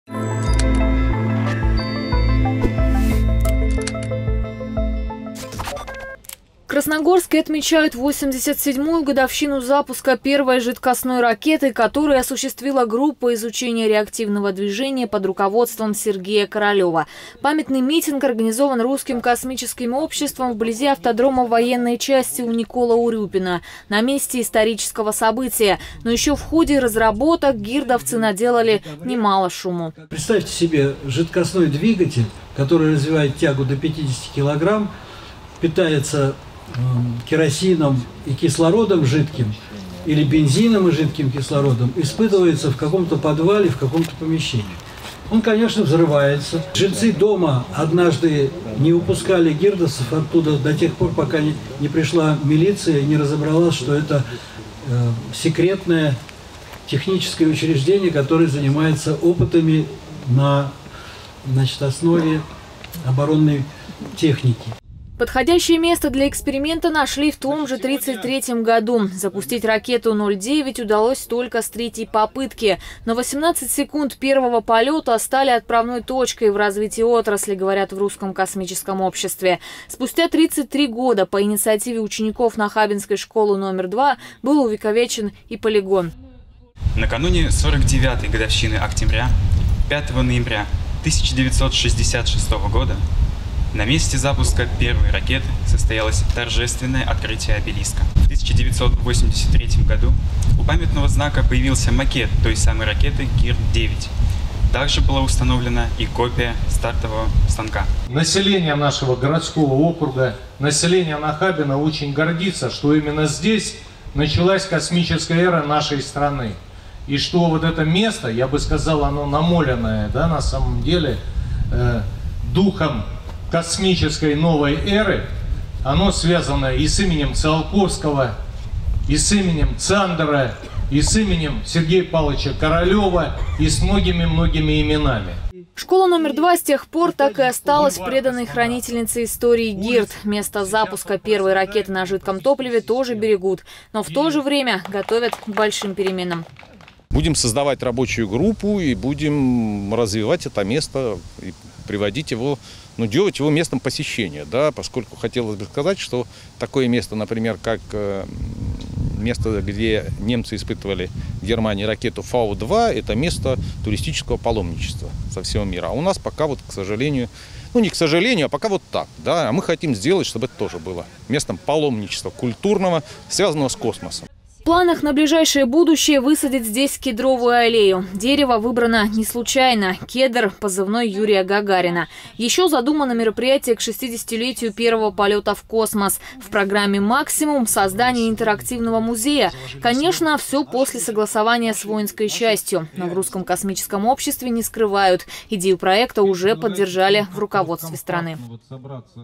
КРТВ. 17 августа - день в истории космоса и Красногорска
В округе отметили 87-ую годовщину запуска первой жидкостной ракеты